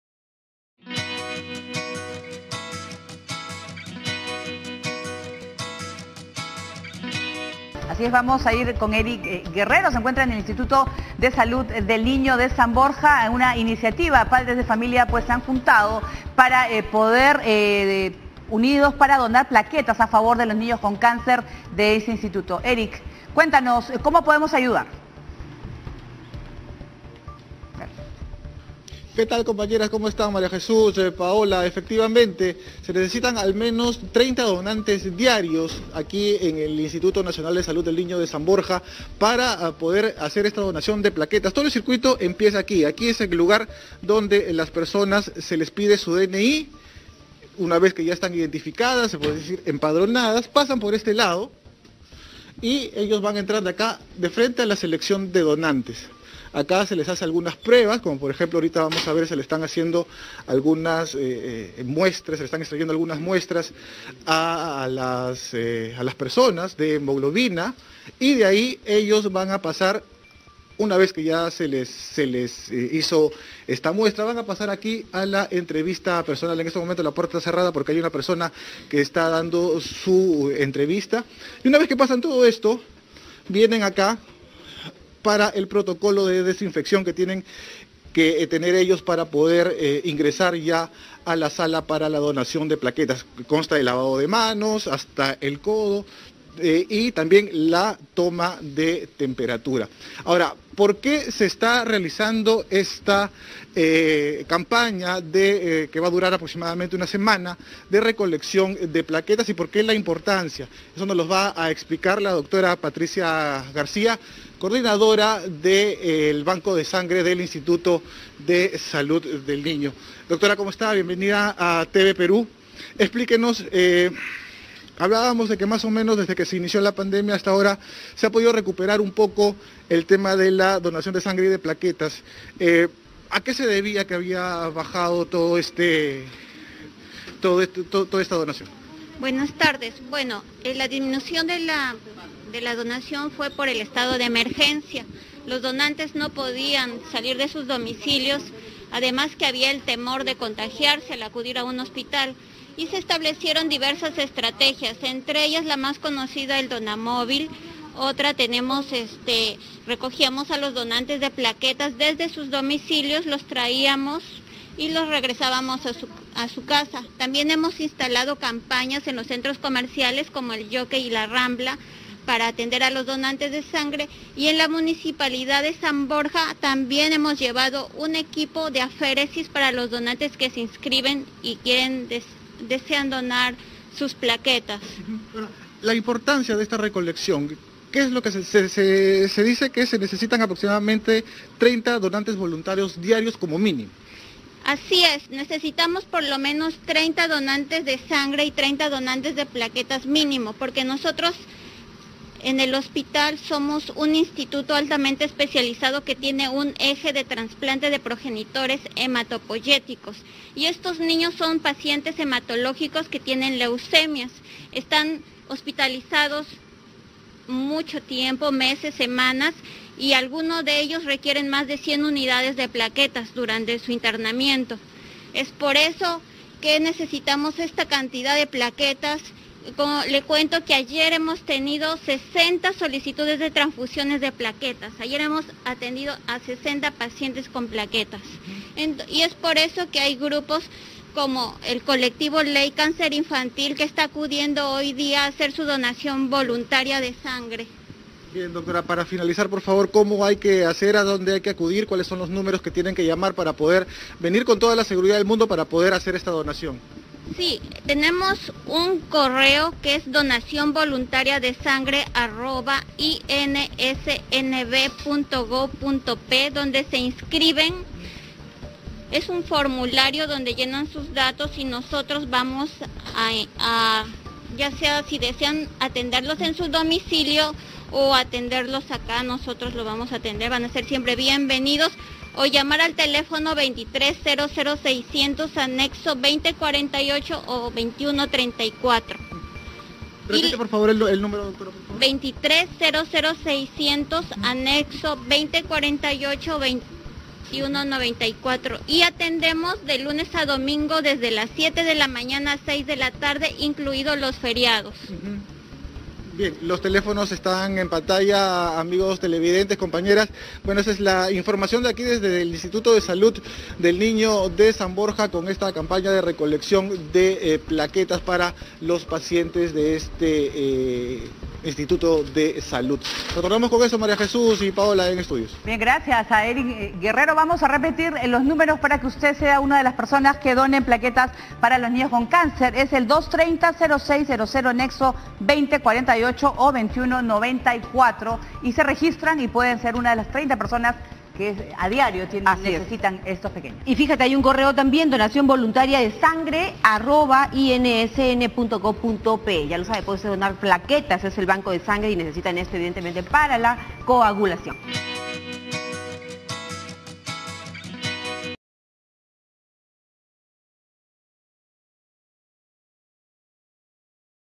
Padres se unen para donar plaquetas a niños con cáncer - INSN San Borja
En vivo. Desde el INSN de San Borja, se informa que padres del Colectivo Ley Cáncer Infantil y voluntarios de la Ong. Corazón Que Late Necesita Donante se unieron para donar plaquetas a niños con cáncer. La coordinadora de Servicio de Banco de Sangre de INSN, Patricia García López detalló que disminución de la donación fue por el estado de emergencia. Indicó que se necesita por lo menos 30 donantes de sangre y 30 donantes de plaquetas.